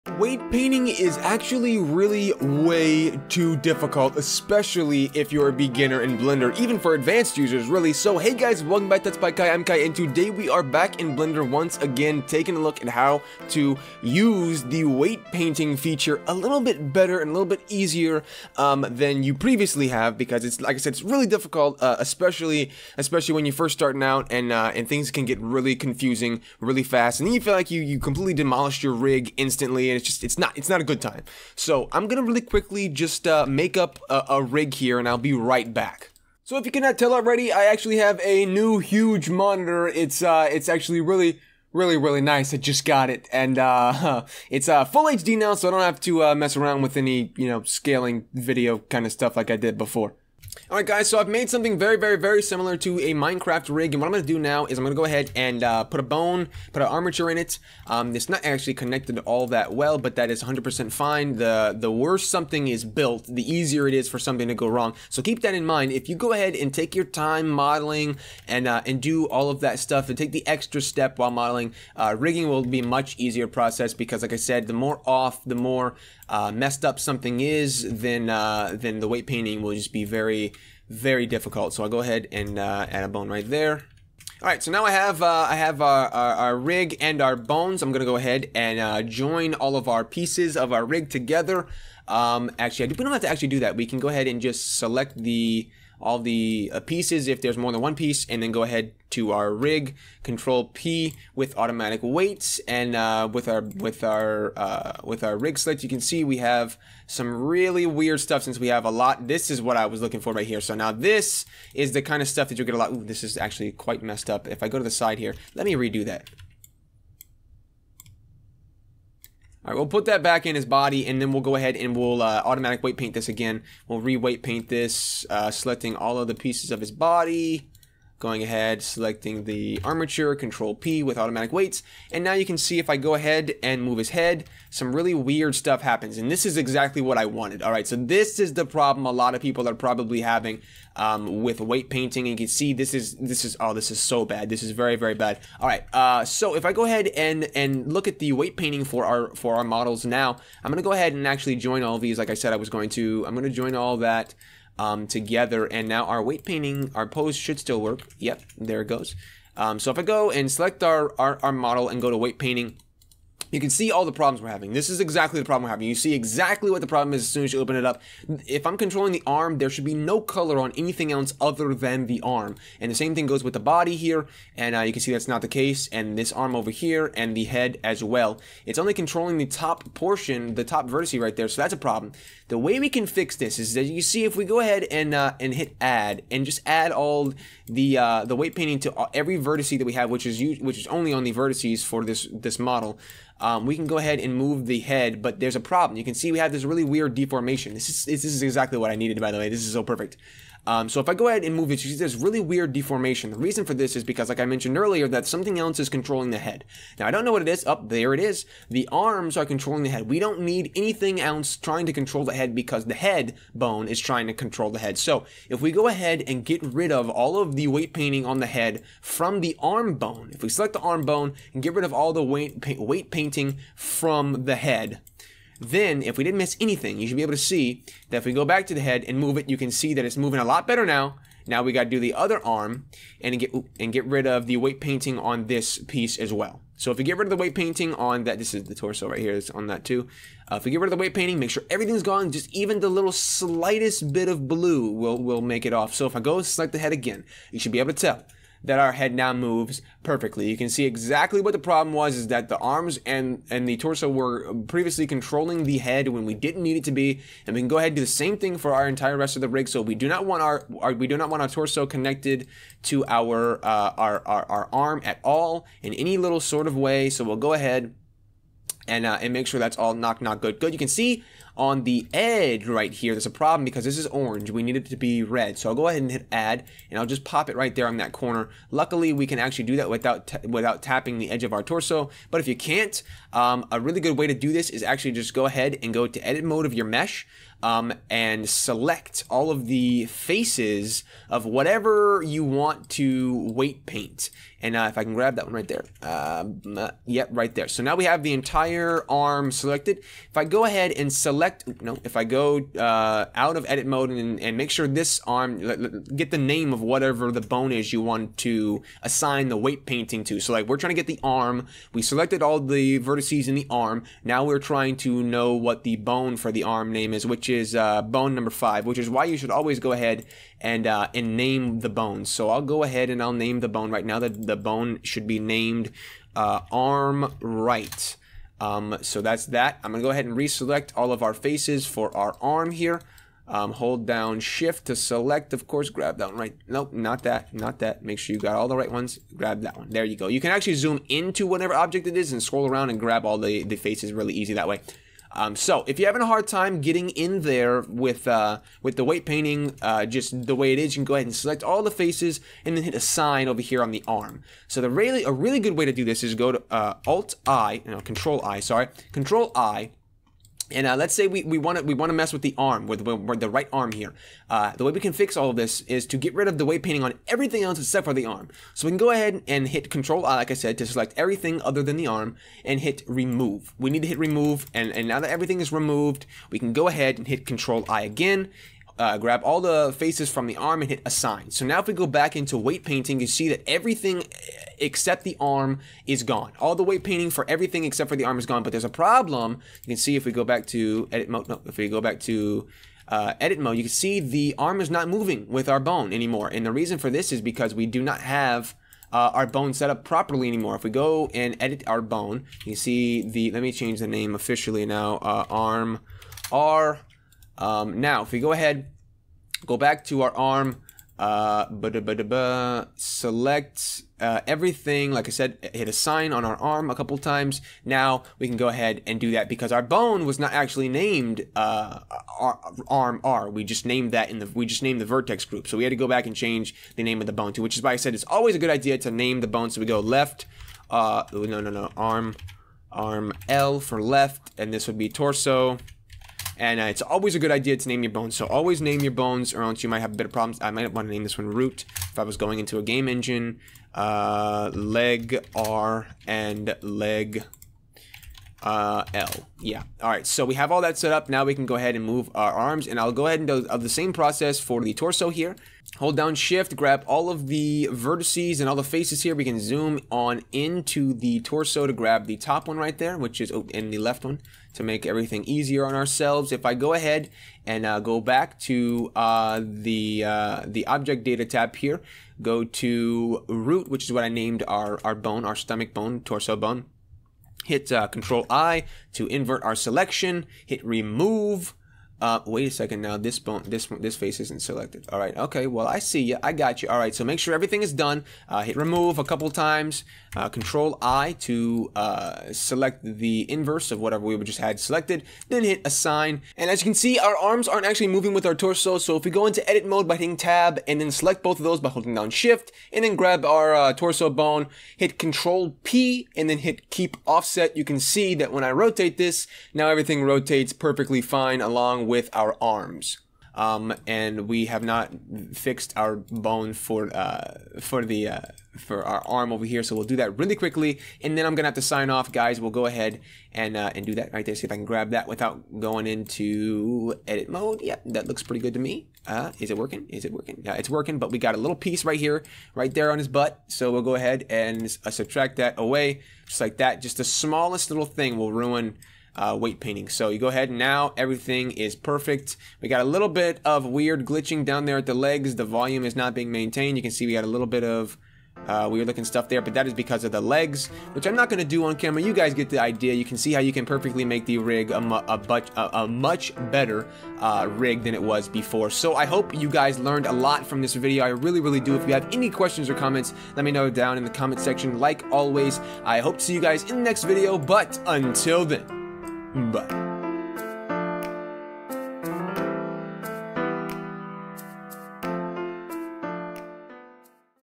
Blender Tutorial - How to Weight Paint in Blender
Today, I share some SUPER easy methods on Weight Painting in Blender, that I know will help out a bunch of you guys and gals!